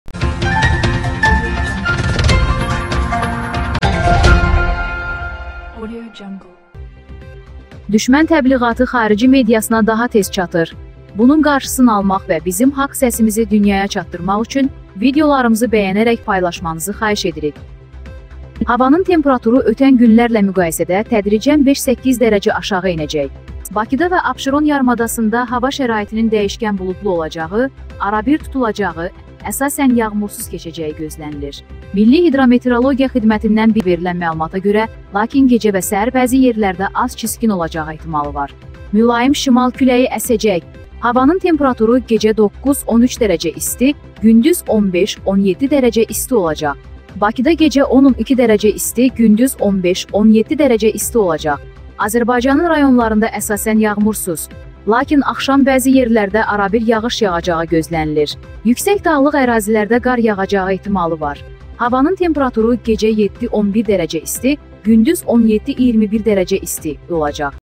Qeyri-sabit hava ilə bağlı XƏBƏRDARLIQ: Temperatur KƏSKİN ENİR, YAĞIŞ, QAR GƏLİR
Bu kanalda abunə olun @TomarBS